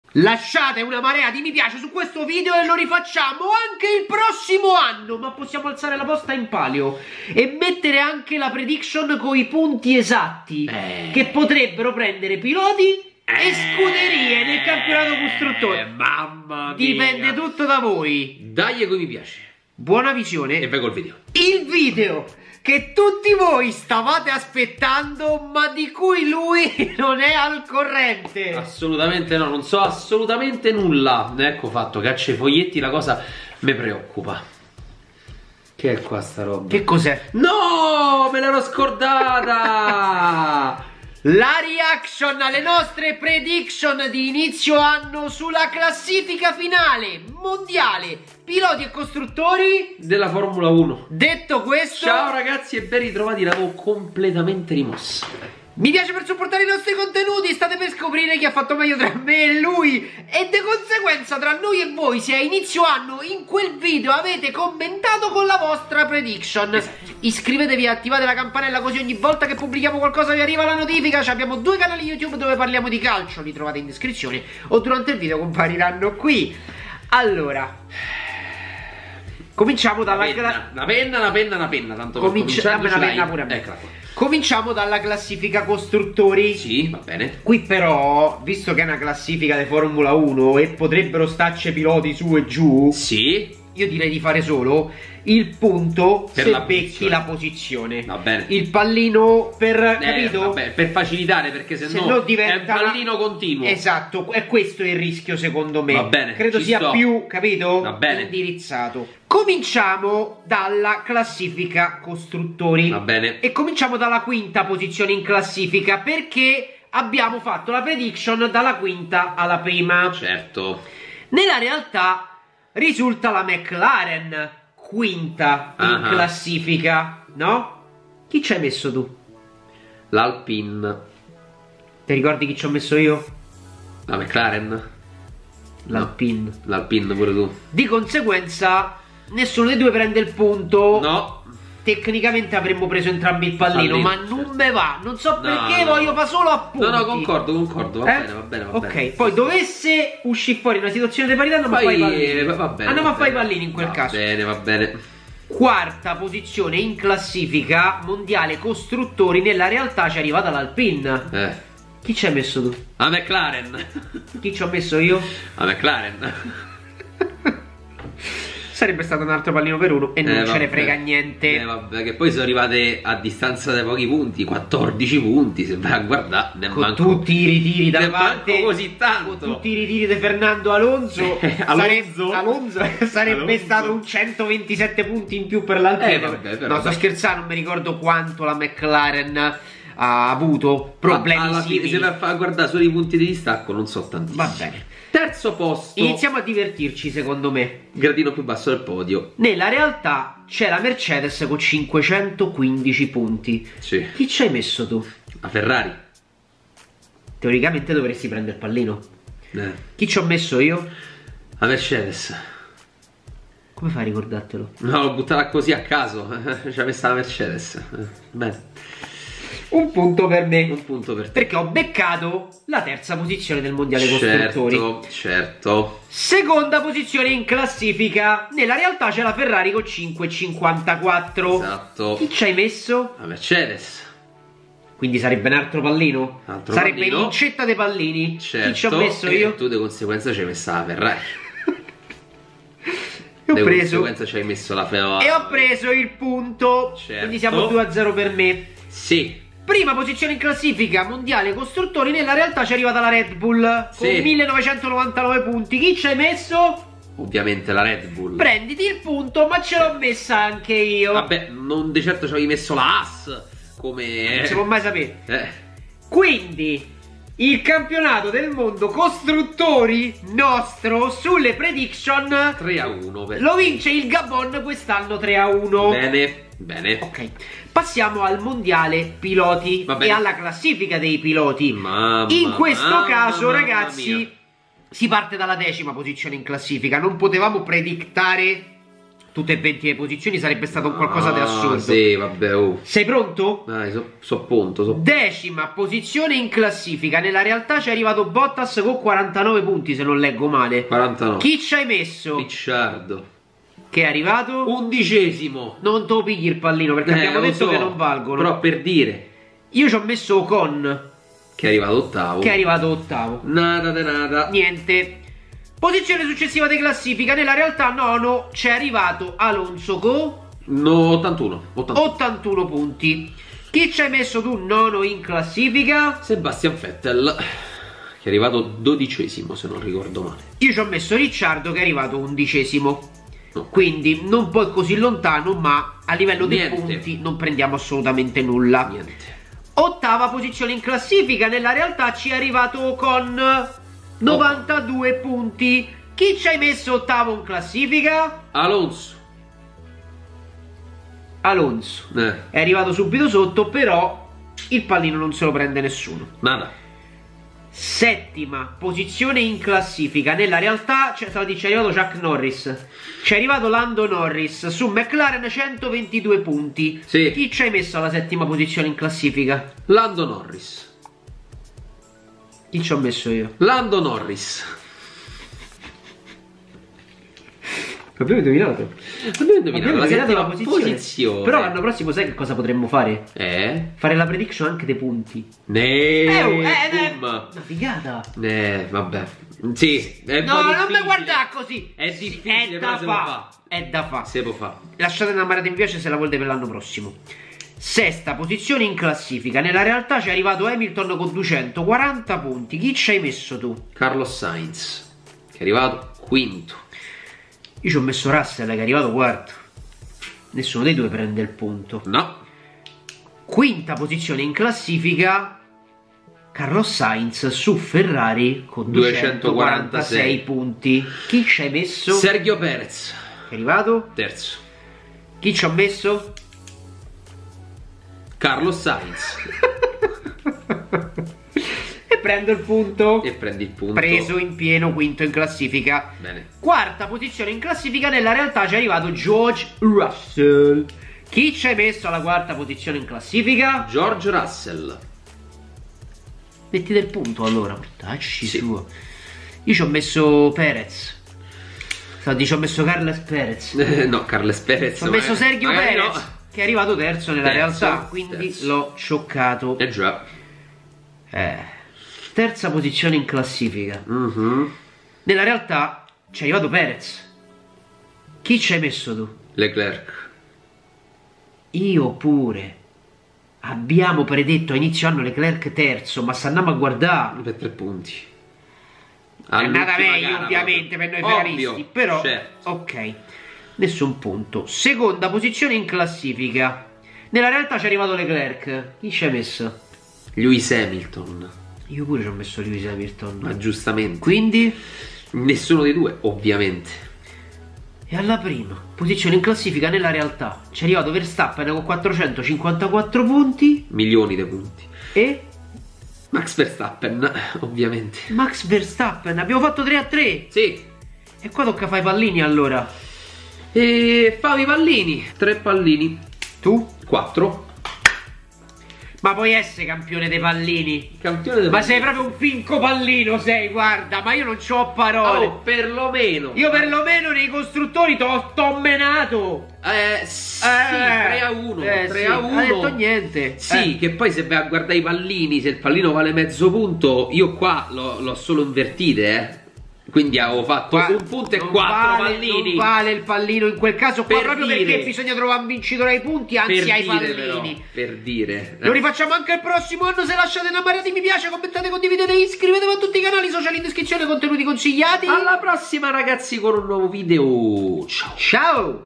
🏎️😱 La REAZIONE alle NOSTRE PREDICTION sulla CLASSIFICA FINALE di FORMULA 1 2022‼️
Ecco la reazione alle nostre prediction sulla classifica finale di Formula 1 2022! (Campionato piloti e costruttori)